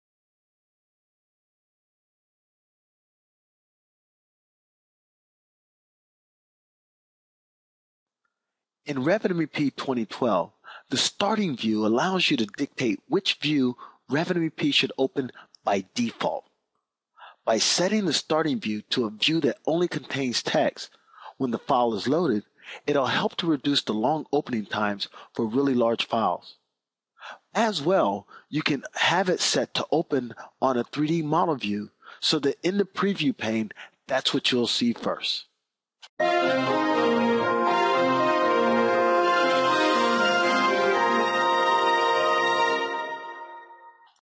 Revit MEP 2012: Starting Views
New in Revit MEP 2012!  Learn to set a default startup view in Autodesk Revit MEP 2012.